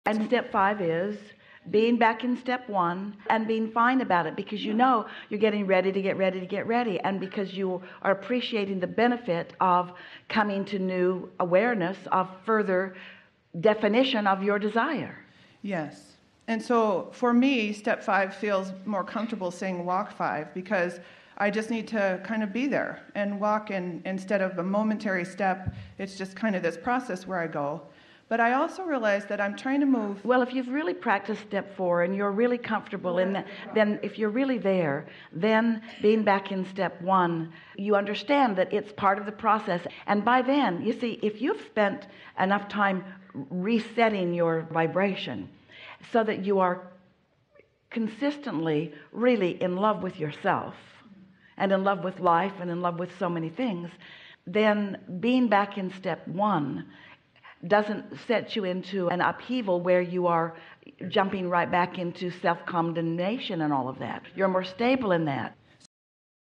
Abraham Hicks: Being Back in Step 1 and Resetting Your Vibration
Topic targeted discussions about creating your own reality and law of attraction from Abraham Hicks.  Love the Abraham material!